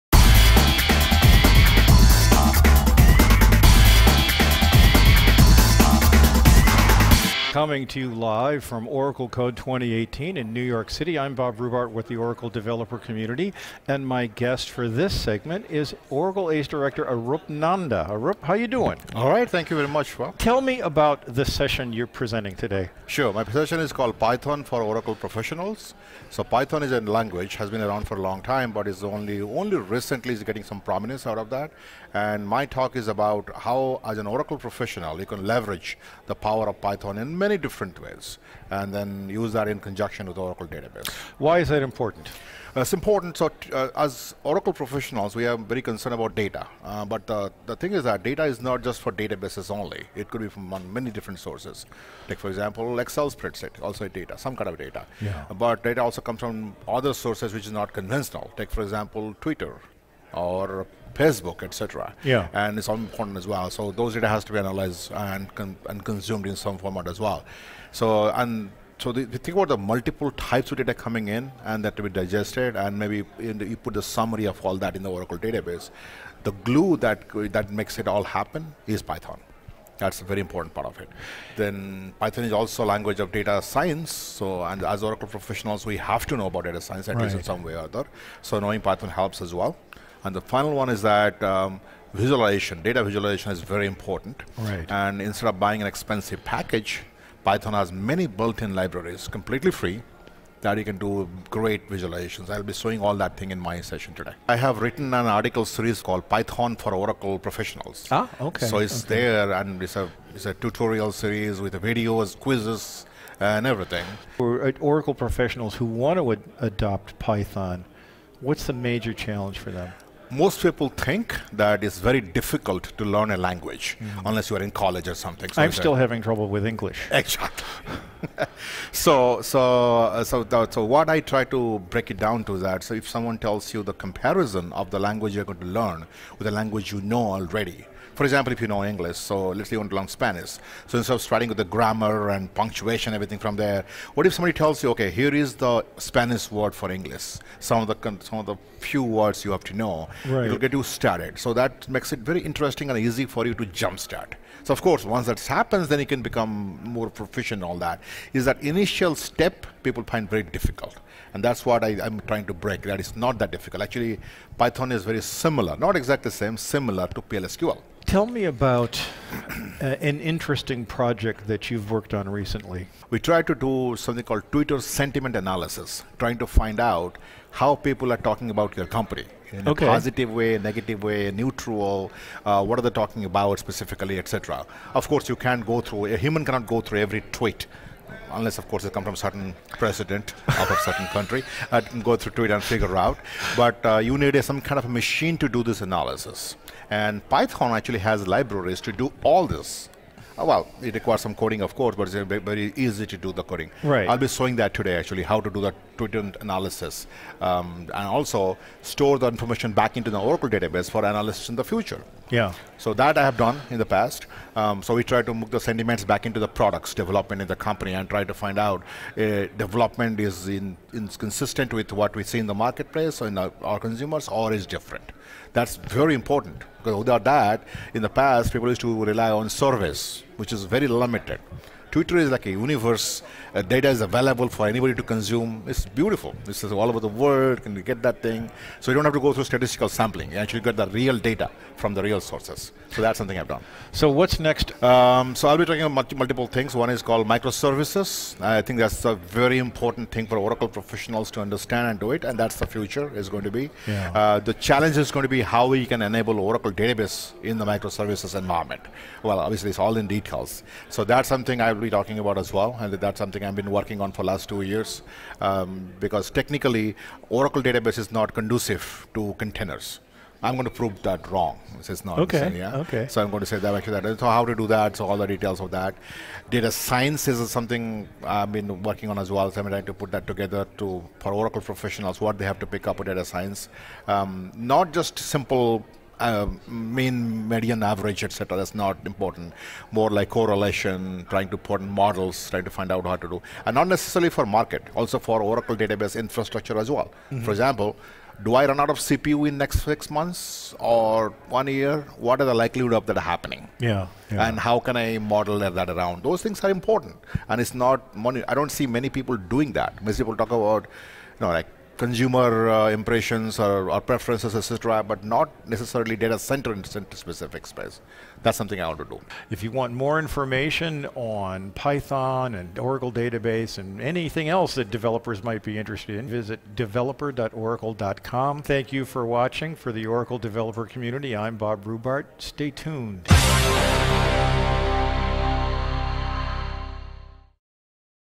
Leveraging the Power of Python
Python has much to offer database developers, and the transition from PL/SQL is easier than you might think. Get the details in this interview with award-winning Oracle Database technologist, architect, and author Arup Nanda.

Also check out Arup's tutorial:
"Introduction to Python for PL/SQL Developers - Full Article Series"